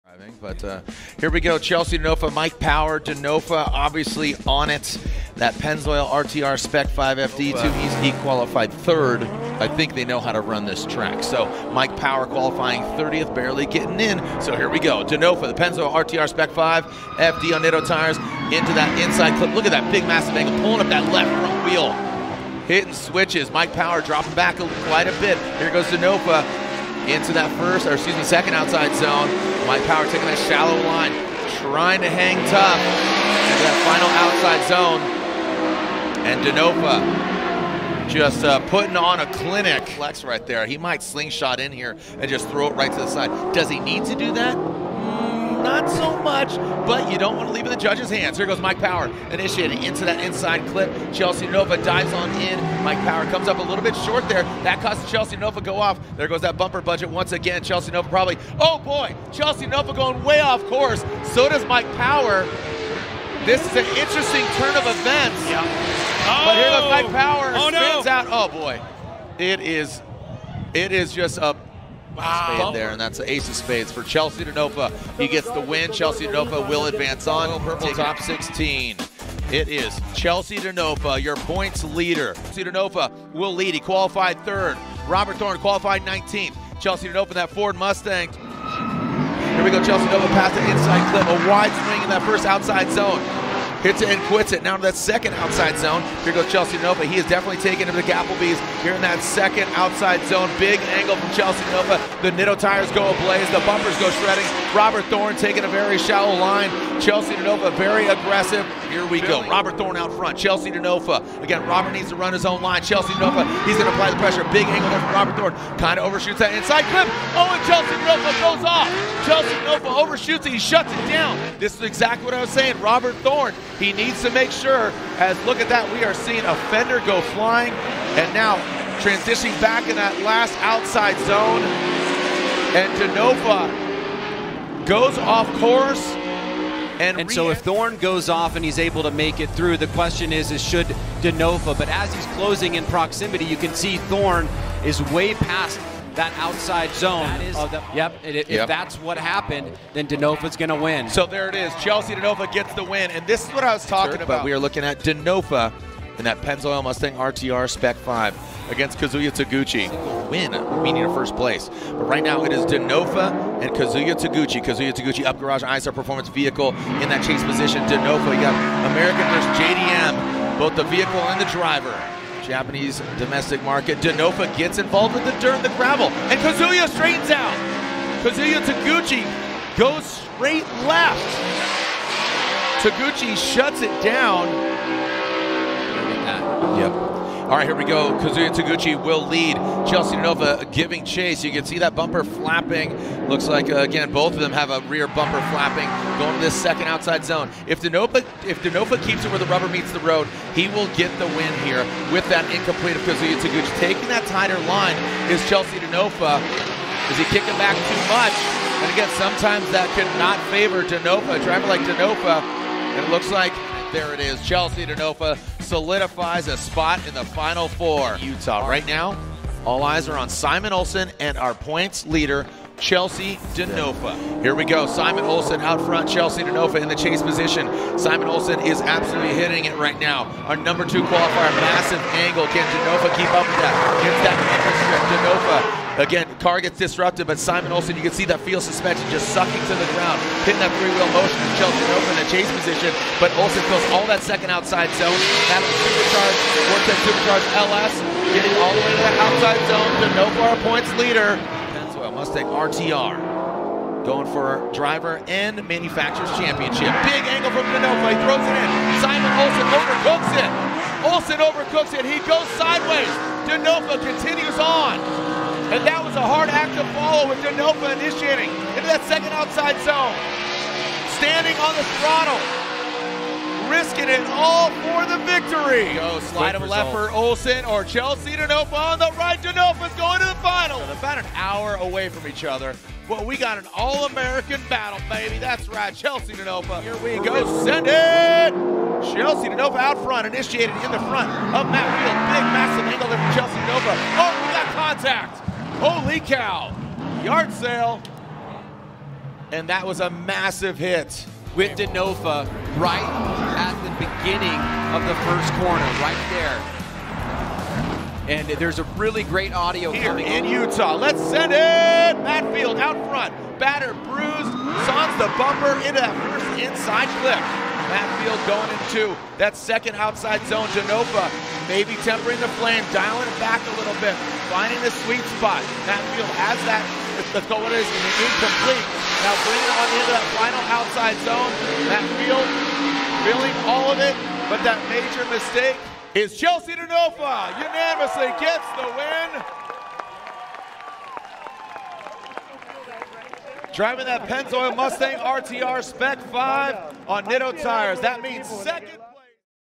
CHELSEA DENOFA Get the 3rd Victory on Formula Drift 2023 RD7 (Grantsville)
Versus:
TOP 32 - Mike Power
TOP 16 - Robert Thorne
TOP 8 - Kazuya Taguchi
TOP 4 - Simen Olsen
Final - Matt Field